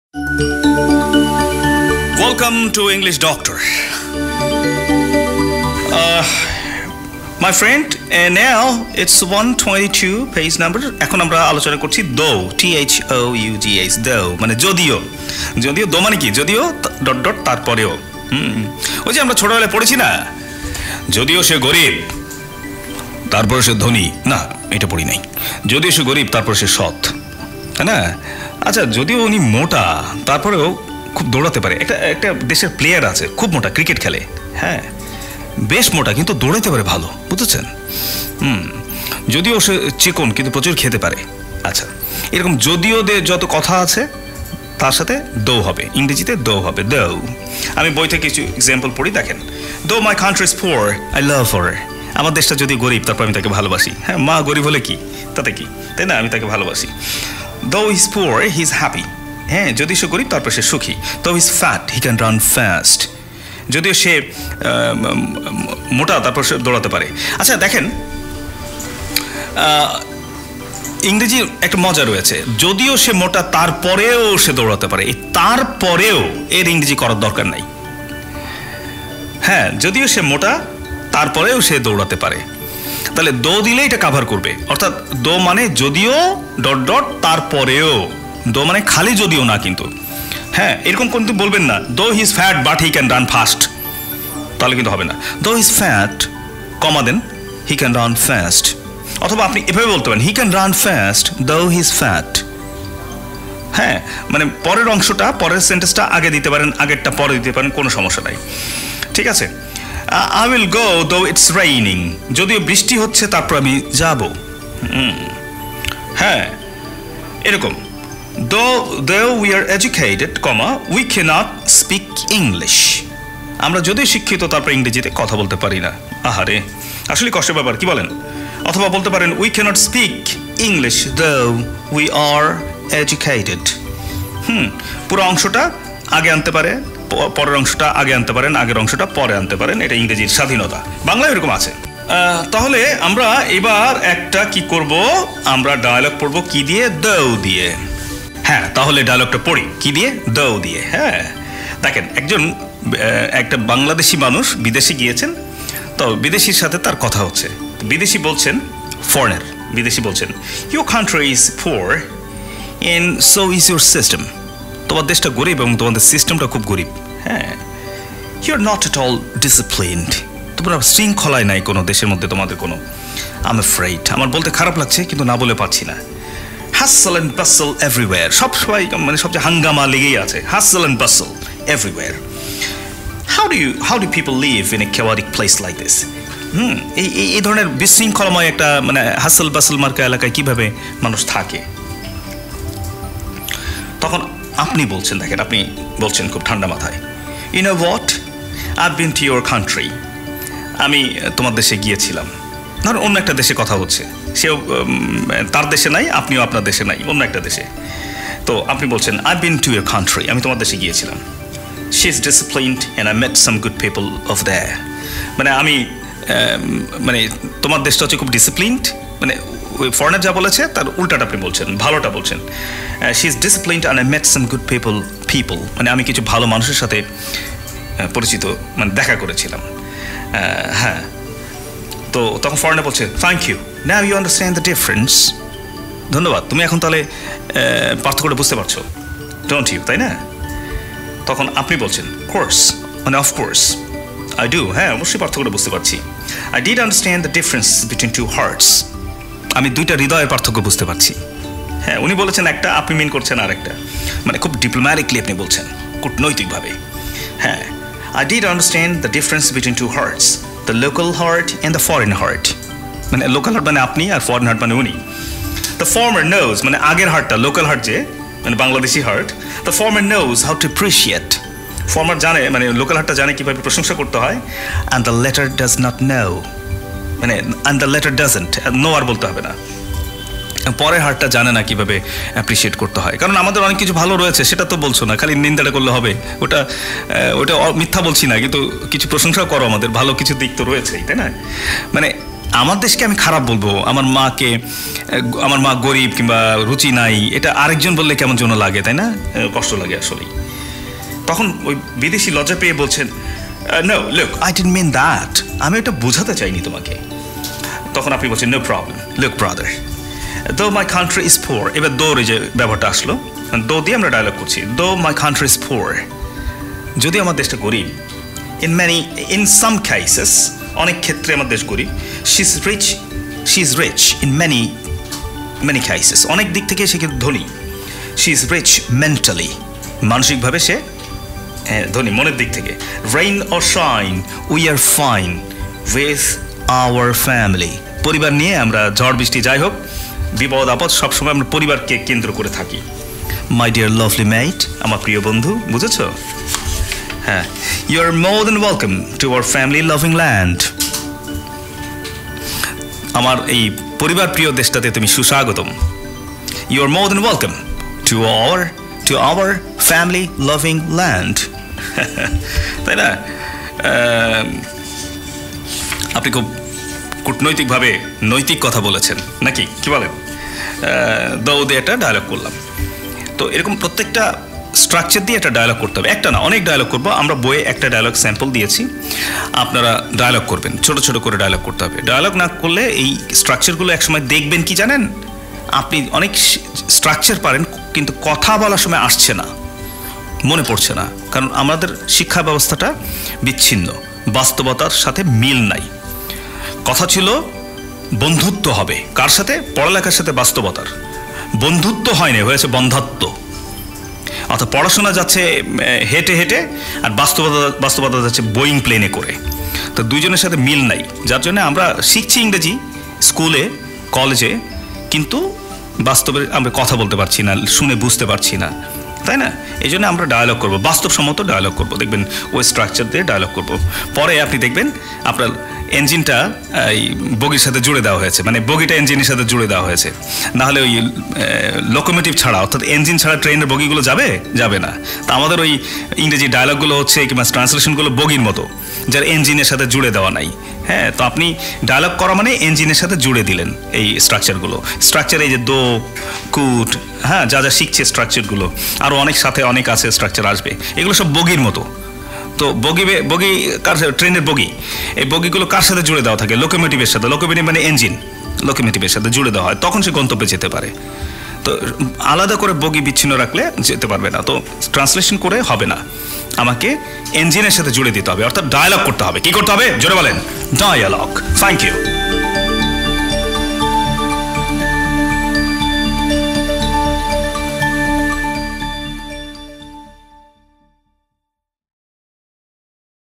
Though | English Doctor | Page : 122 | TESOL Bangladesh
A Note on "Though" from "English Doctor"
Prompt: Provide a description of the title "Though" from the book "English Doctor" by TESOL Bangladesh, page 122.

Response:

Unfortunately, without access to the specific content of page 122 in "English Doctor," I cannot provide a definitive description.

However, based on the common use of the word "though," I can offer some potential interpretations:

Possible Interpretations:
Conjunctions: The page might be discussing the use of "though" as a conjunction to introduce a contrasting idea or concession. For example, "Although it's raining, I'll still go for a walk."
Adverbs: The page could focus on the use of "though" as an adverb, often placed at the end of a sentence to express a slight contrast or concession. For example, "He's a good student, though he can be a bit lazy."
Sentence Structure: The page might explore how to use "though" effectively in sentence structure, including its placement and the different types of clauses it can introduce.